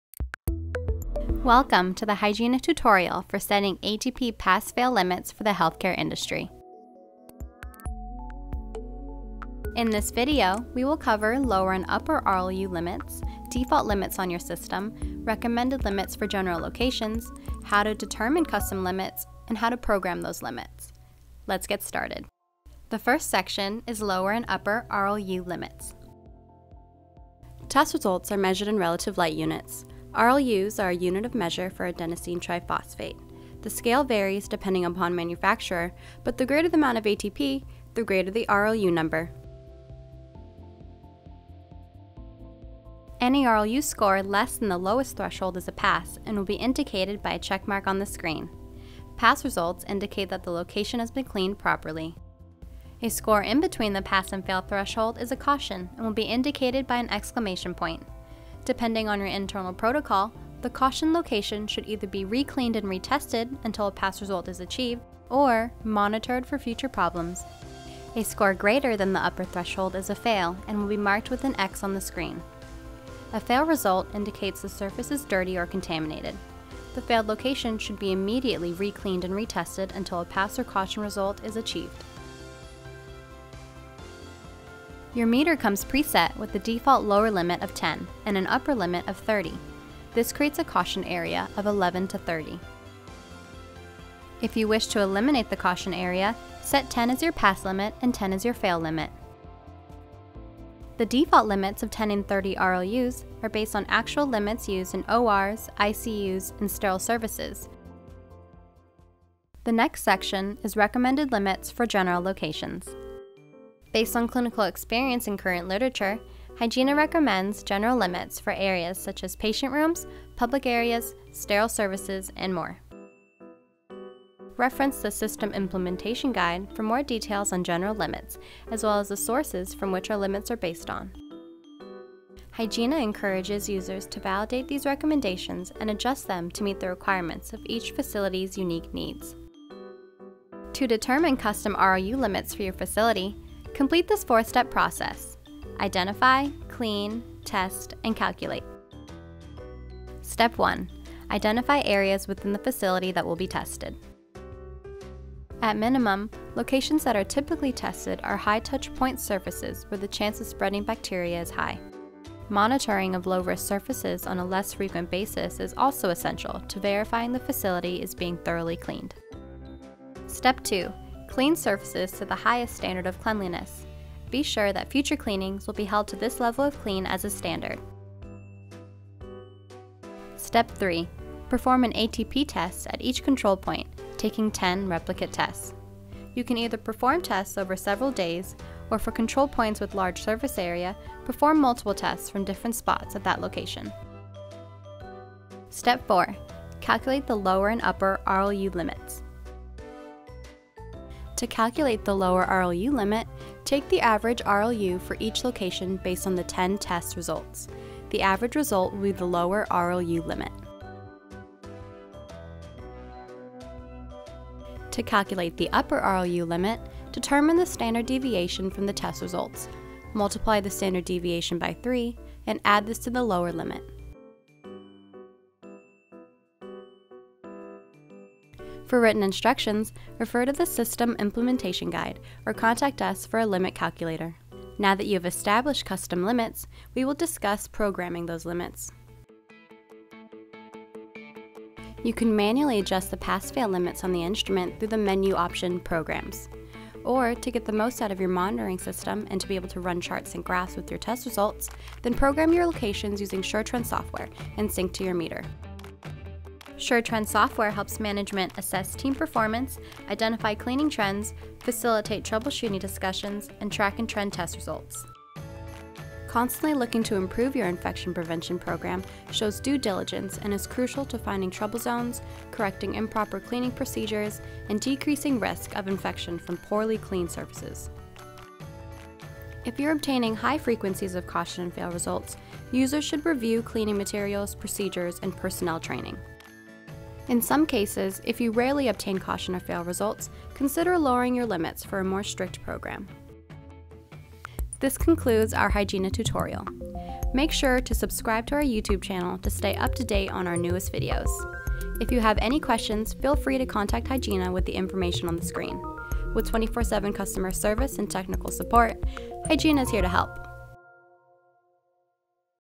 Setting ATP Pass & Fail Limits for Healthcare Industry
Turn on closed captioning for translations!
This video will guide you through setting up and understanding Pass & Fail RLU limits on Hygiena ATP Cleaning Verification System for Healthcare applications. For more information about establishing and setting limits on your meter, . There, you can download resources to guide you through set up.

With 24/7 customer service and technical support, Hygiena is here to help!